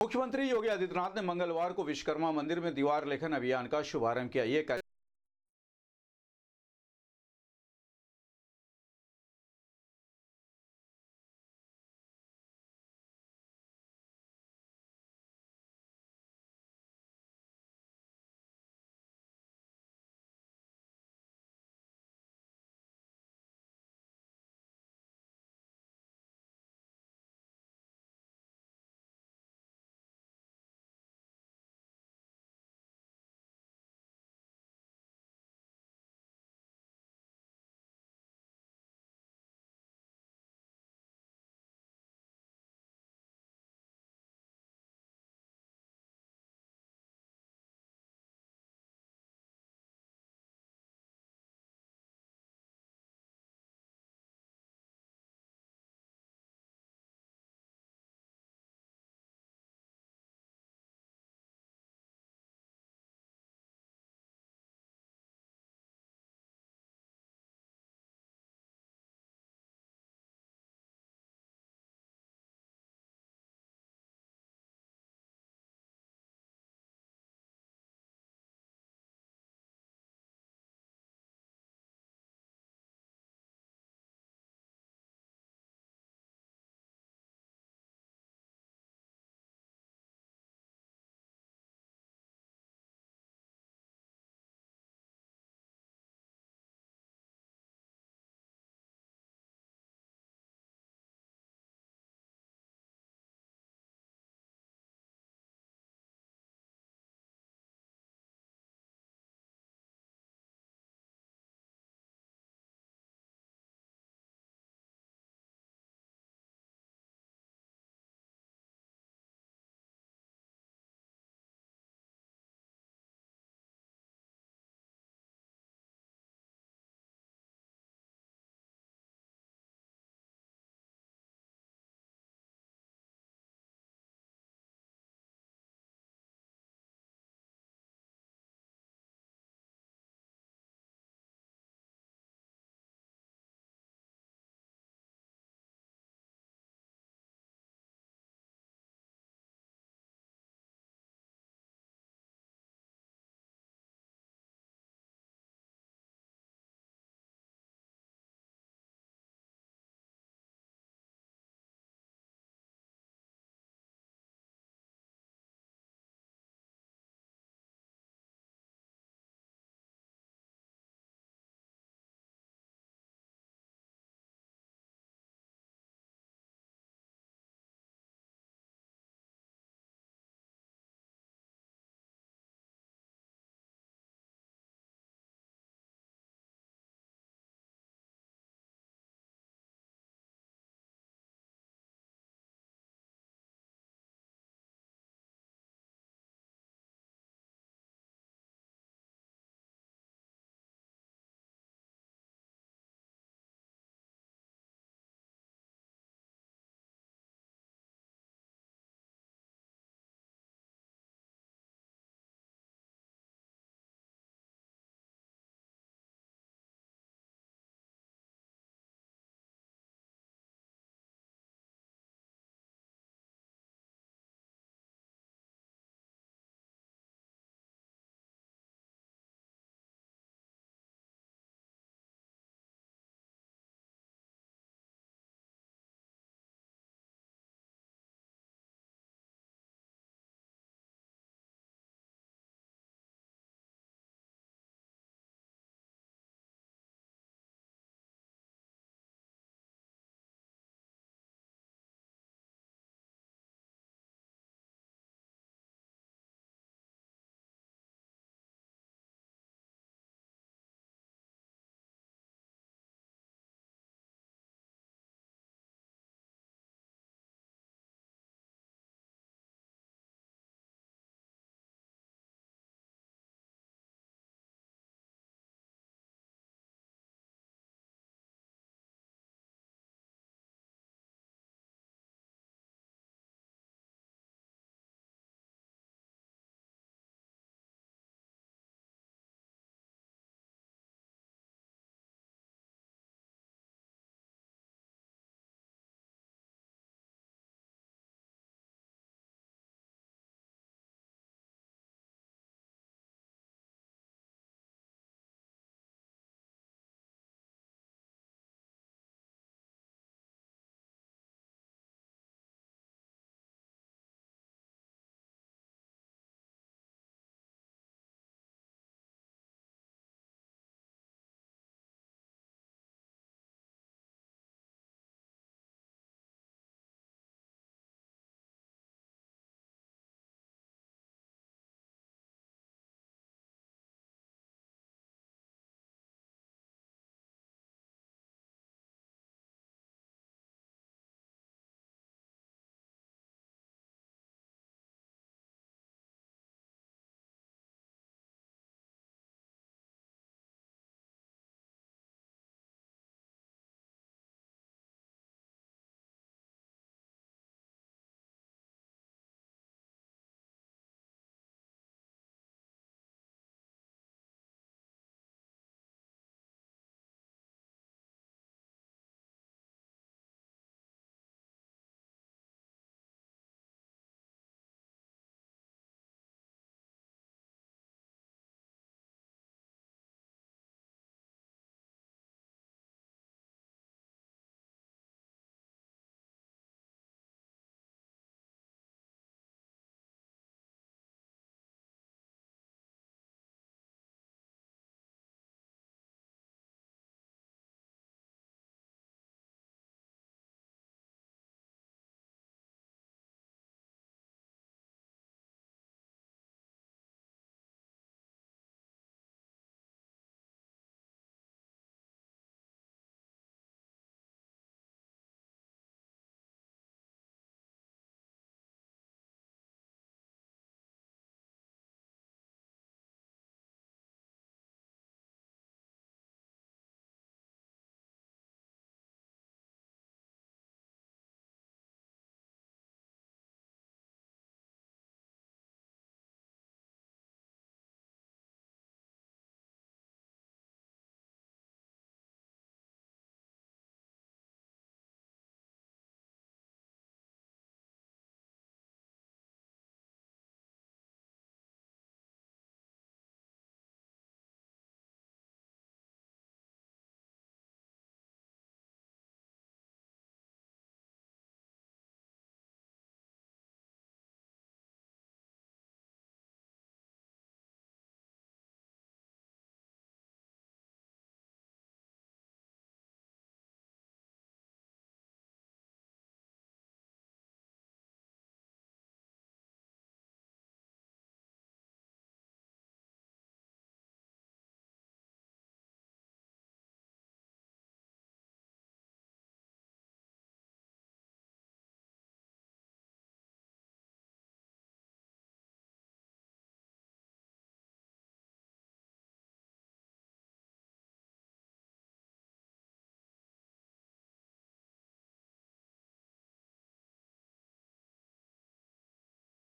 मुख्यमंत्री योगी आदित्यनाथ ने धर्मशाला स्थित विश्वकर्मा मंदिर में दीवार लेखन अभियान का किया शुभारंभ
मुख्यमंत्री योगी आदित्यनाथ ने धर्मशाला स्थित विश्वकर्मा मंदिर में दीवार लेखन अभियान का किया शुभारंभ,अपने हाथों से बनाया कमल का फूल,कहा अबकी बार चार सौ पार